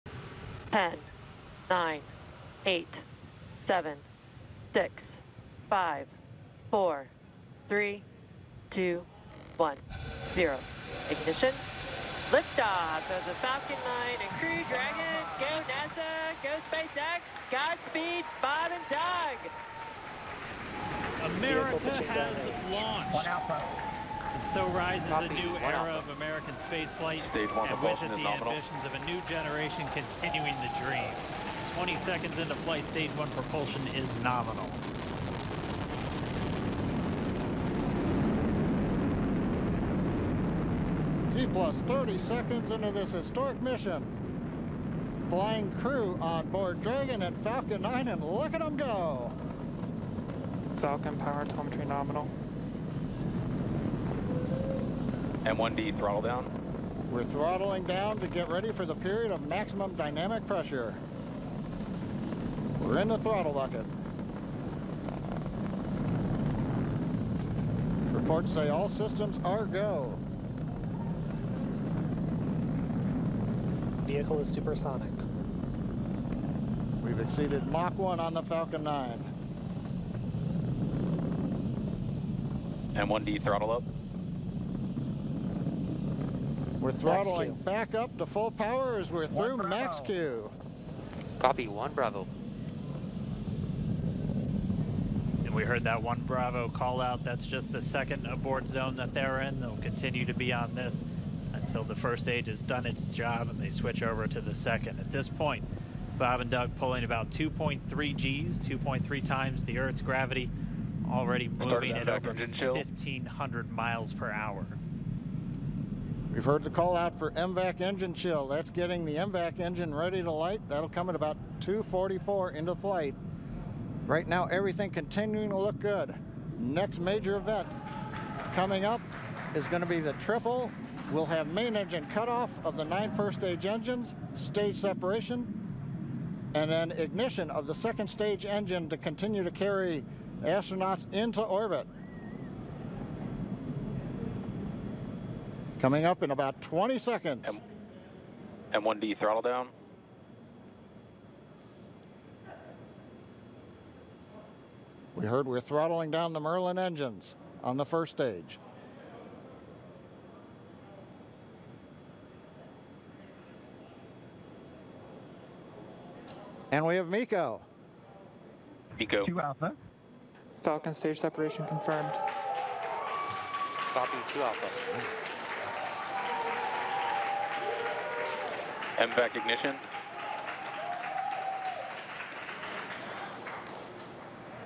NASA, SpaceX launch manned spacecraft in historic mission
SpaceX made history on Saturday after its Crew Dragon spacecraft lifted off for the International Space Station (ISS).

The launch, a partnership with NASA, is the first time a privately built spacecraft carried astronauts into orbit.

The Falcon 9 rocket, which detached from the spacecraft after expending its fuel, was also confirmed to have successfully landed on a drone ship in the ocean.

The launch took place at Launch Complex 39A at NASA’s Kennedy Space Center in Cape Canaveral, Fla.